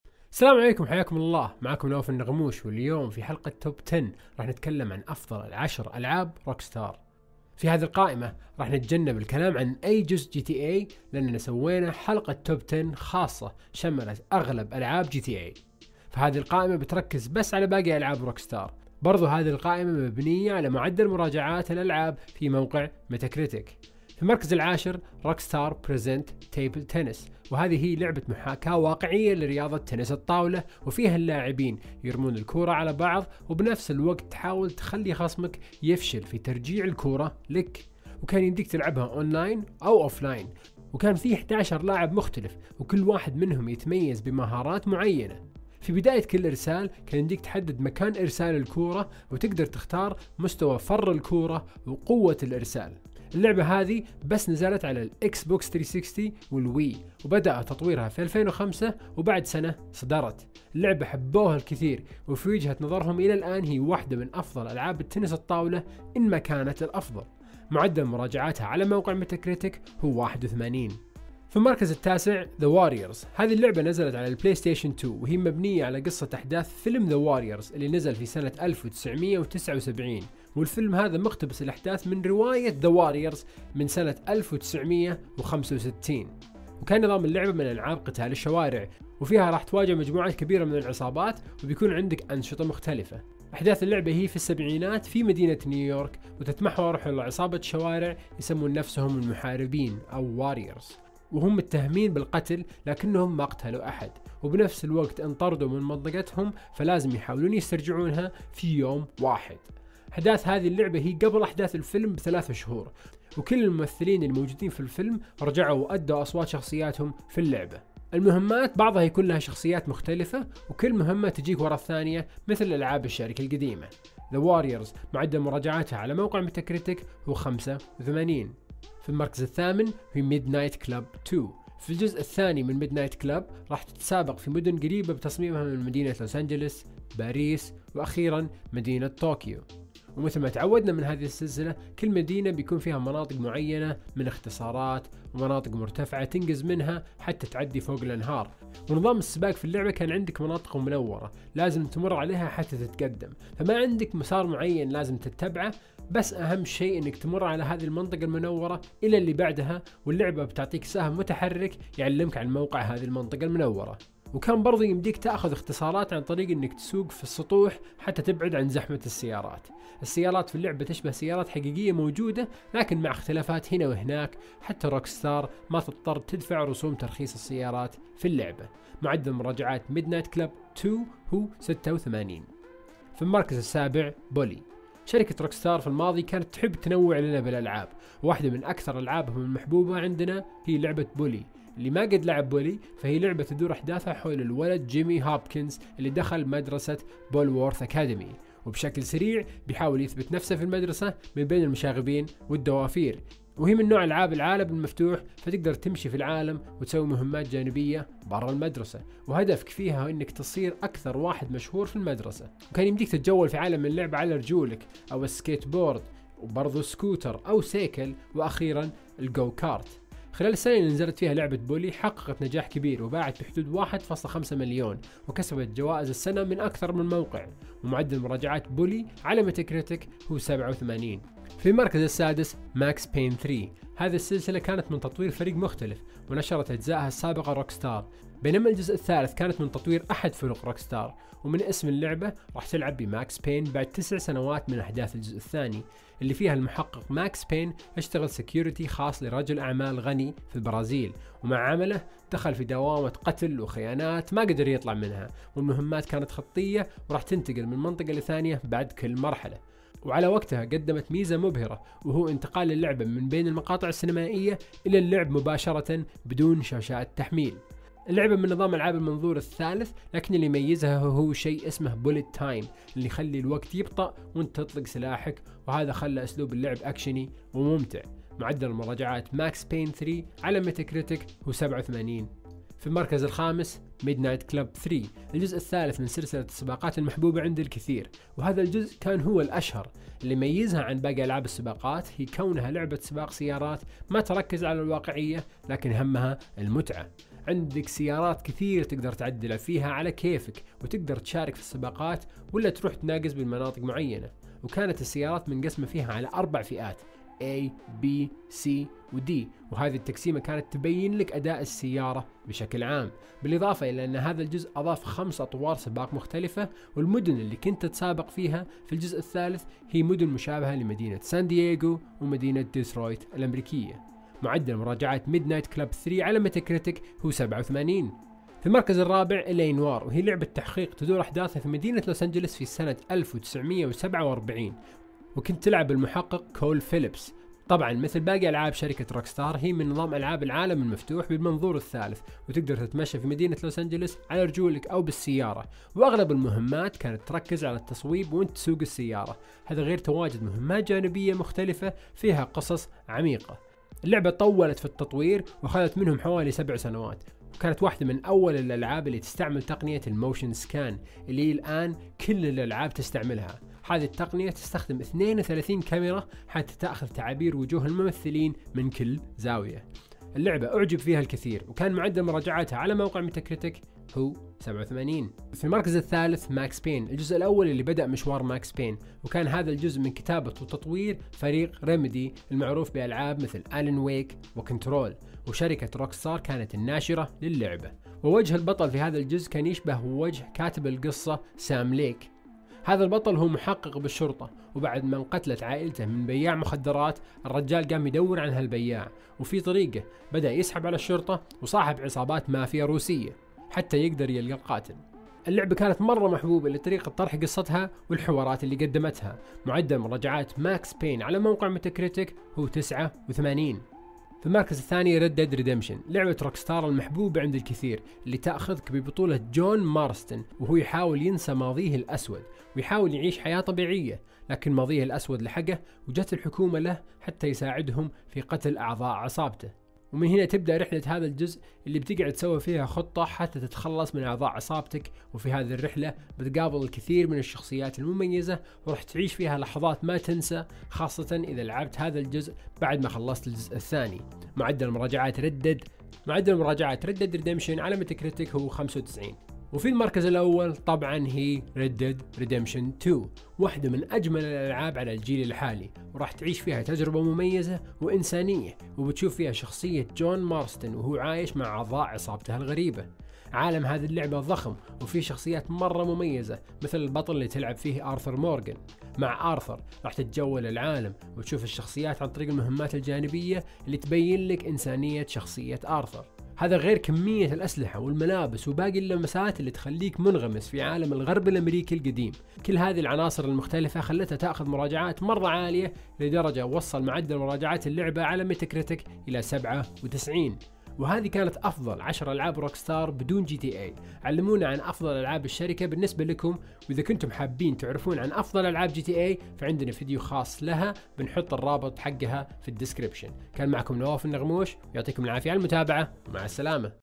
توب 10: أفضل ألعاب روكستار
في هذا الفيديو بنطرح أفضل عشر العاب روكستار
اذا تبون تشوفون أفضل العاب جراند تقدرون تشوفونها في هذا الرابط
حمل تطبيقنا عالهواتف الذكية!